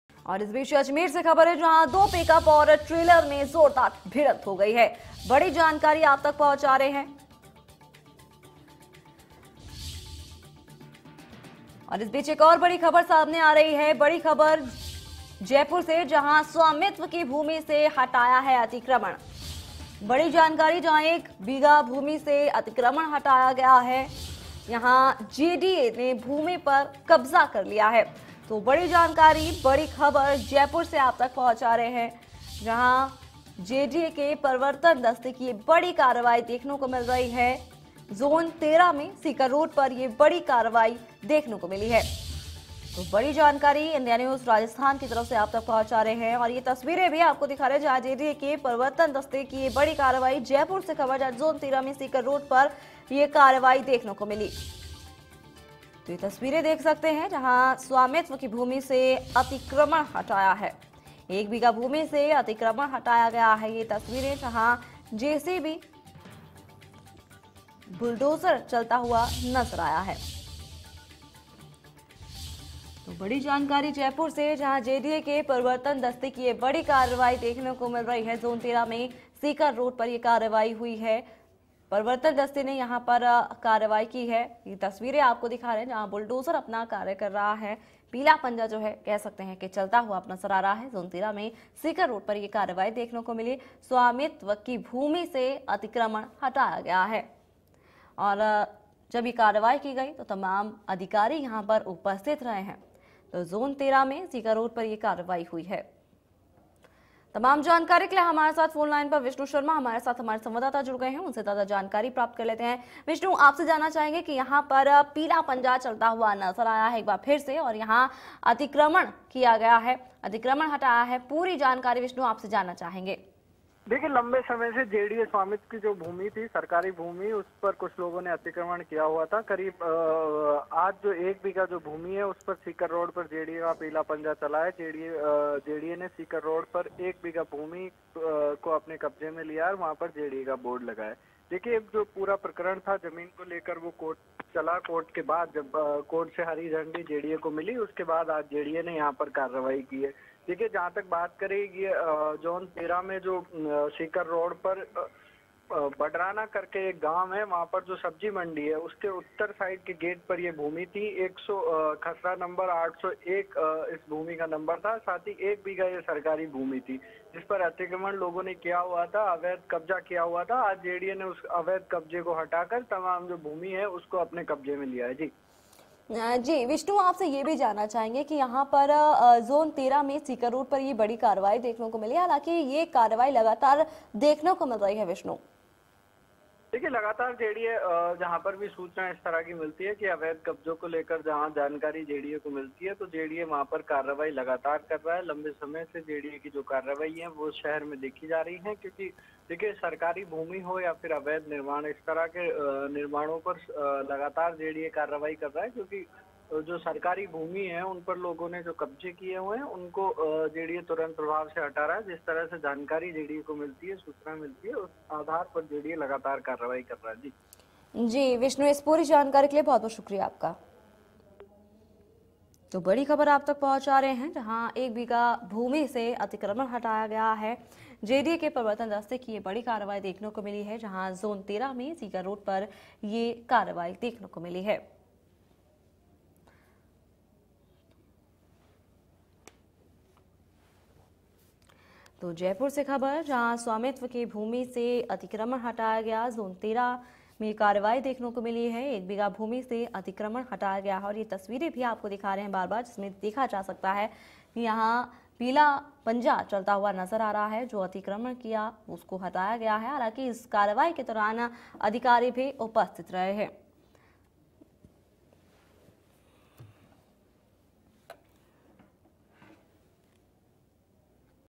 Jaipur News : JDA के प्रवर्तन दस्ते की कार्रवाई | Hindi News | India News Rajasthan
Jaipur News : JDA के प्रवर्तन दस्ते की कार्रवाई | Hindi News

जोन-13 में स्पीकर रोड पर की कार्रवाई, JDA ने भूमि पर लिया कब्जा